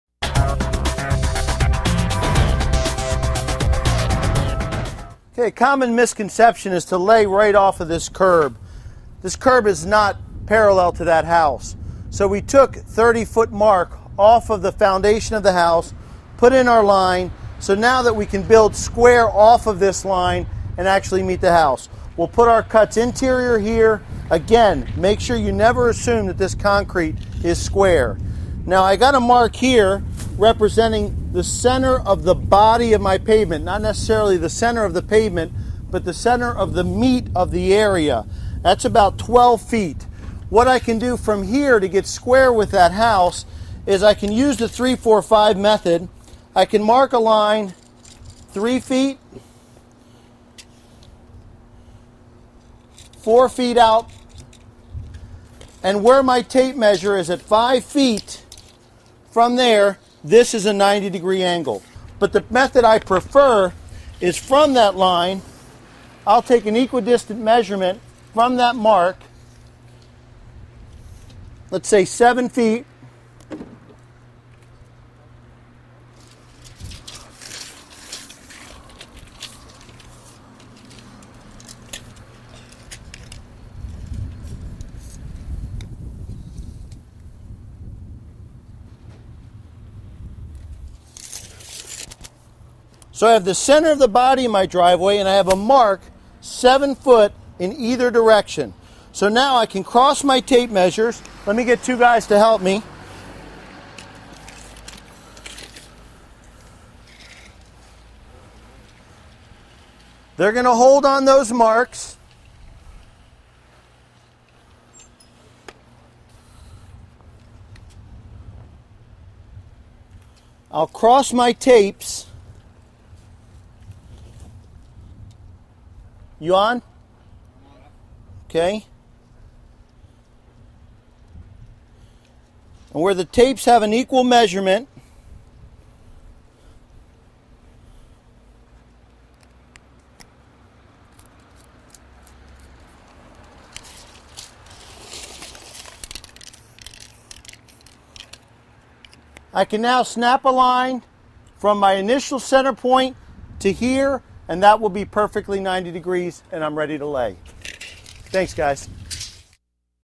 Getting Square
An example of how to cross measuring tapes in order to lay your pavers in a 90-degree angle.